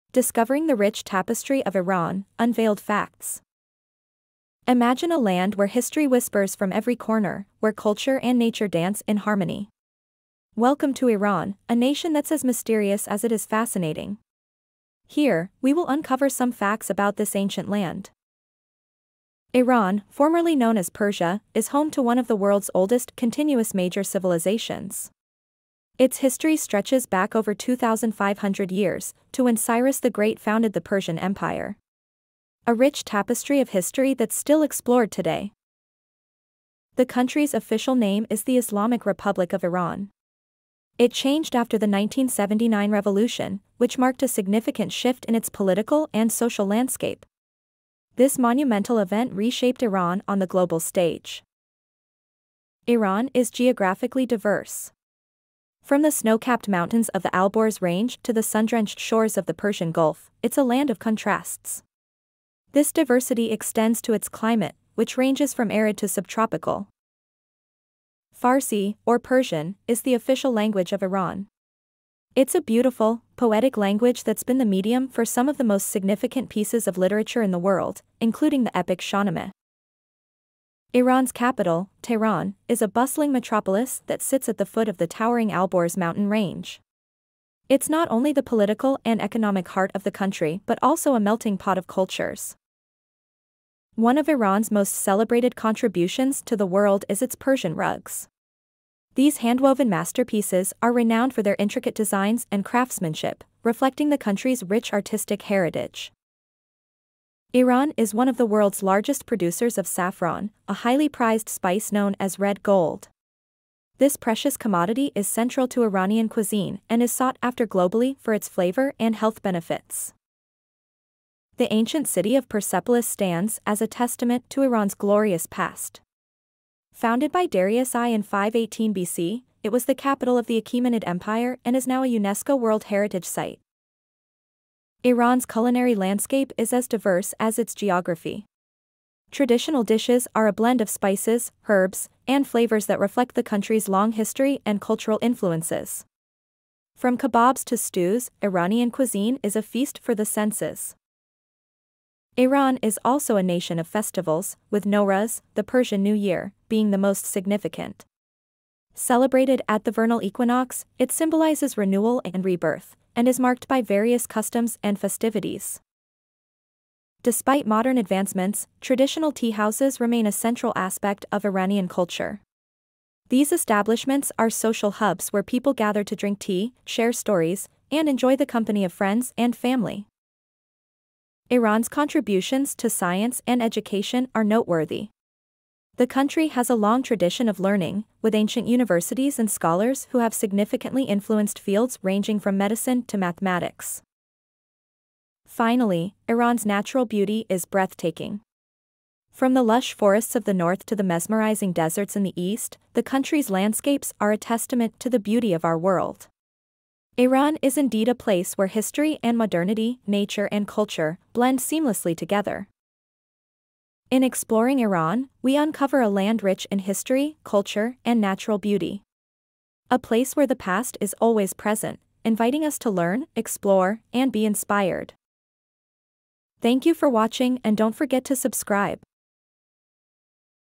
Facts about Iran 🇮🇷
Embark on a captivating journey through the rich tapestry of Iran, a land steeped in history, culture, and tradition. Join us as we unveil fascinating facts about this enchanting country, from its ancient landmarks and vibrant cities to its culinary delights and diverse heritage. Whether you're a history buff, a travel enthusiast, or simply curious about the world, our videos will offer a glimpse into the beauty and complexity of Iran.